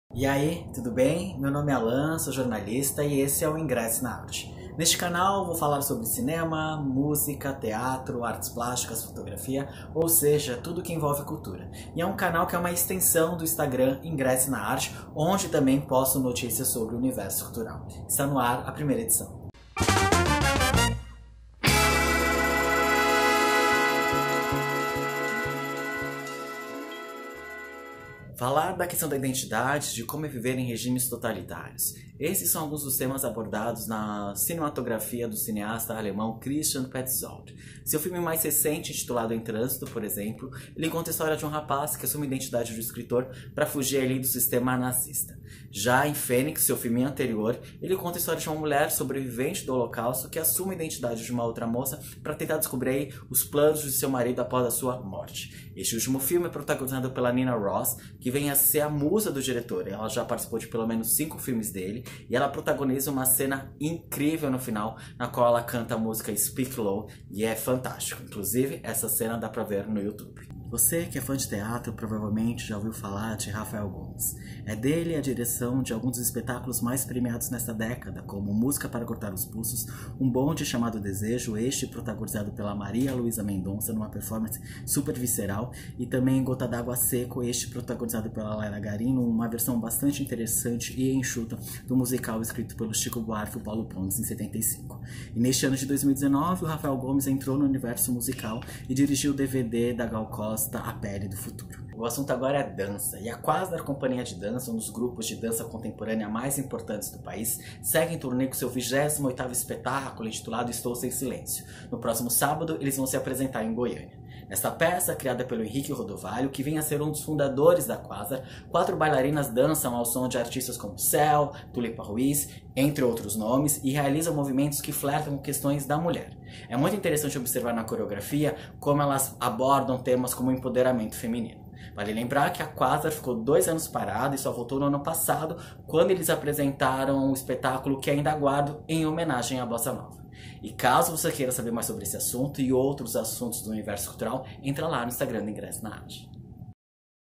Programa 1: Cinema alemão, o teatro de Rafael Gomes e Quasar
Neste vídeo, falo sobre os filmes "Em Trânsito" e "Phoenix", do cineasta alemão Christian Petzold; o diretor teatral Rafael Gomes, responsável por montagens como "Um Bonde Chamado Desejo"; e a companhia de dança de Goiânia Quasar.

On this video, I talk about some Christian Petzold's movies; the theatrical director Rafael Gomes and the Quasar Dance Company. (there are English subtitles)